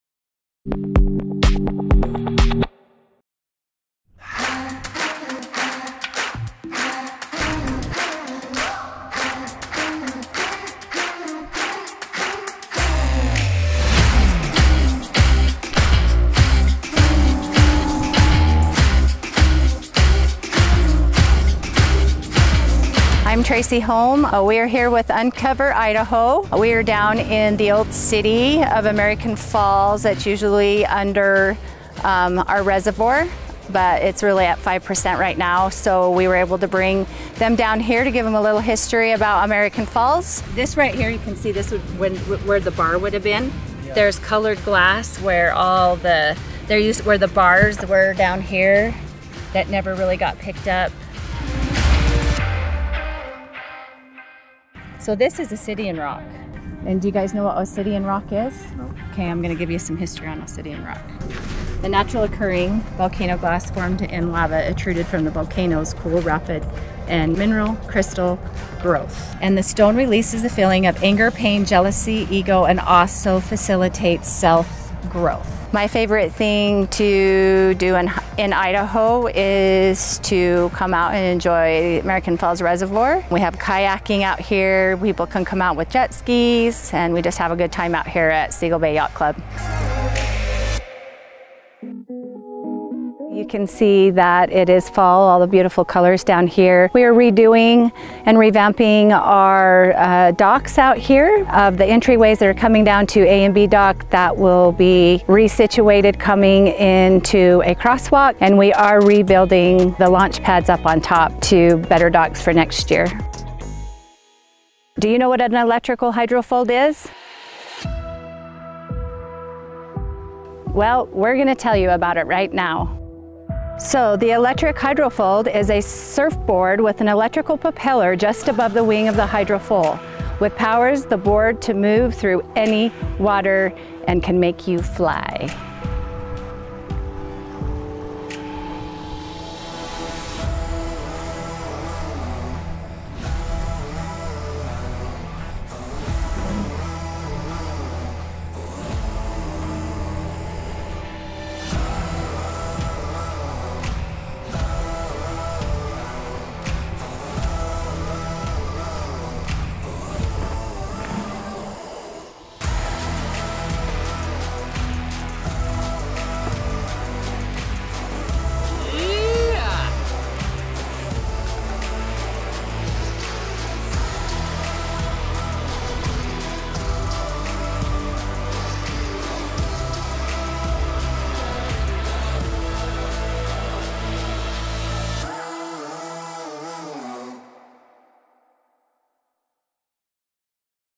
What Lies Below - American Falls Reservoir
Uncover Idaho - What Lies Below - American Falls Reservoir

Idaho is home to over 100 ghost towns, and there is one particular community in southeast Idaho that is unrivaled anywhere else…an underwater one!  The town of American Falls was once a thriving  hub located on the west bank of the Snake River, but when the town was entirely moved during the largest government relocation of its time, the town was moved in its entirety to avoid flooding by the nearby dam.  What lies below the muddy depths is amazing.

Every so often, water levels recede enough for the remnants of the town to become visible. Concrete sidewalks carved with the initials of original homeowners, glass from the city’s bars, and stone landscaping all reappear from their watery graves to tell a striking but mysterious tale. Traci Holm from the nearby Seagull Bay Yacht Club and the team from Uncover Idaho captured such a rare opportunity when the water was low.  You’ll want to check it out.   For the rest of the year, the opaque waters of the American Falls Reservoir provide for kayaking, jet skiing and e-foiling opportunities which UI didn’t shy away from on this visit either.  E-foiling, you say.  YES!  Check it out here.

Make sure to hit the 'Set Reminder' button and we'll see you then.